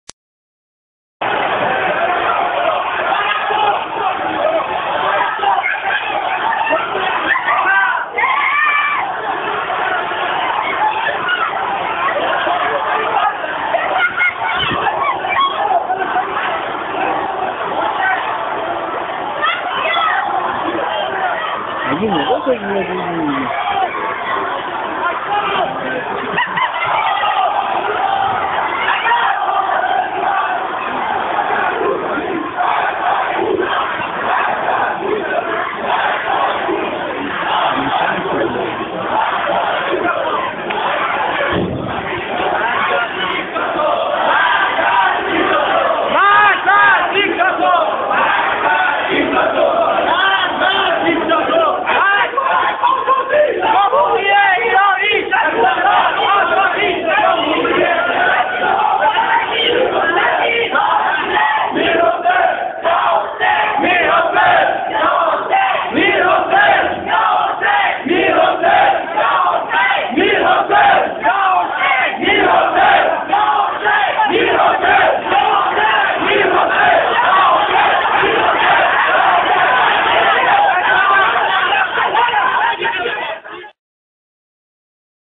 Mashhad Univercity Dec 30///دانشگاه آزاد مشهد- 9 دی
حمله مزدوران بسیجی به دانشجویان دانشگاه آزاد مشهد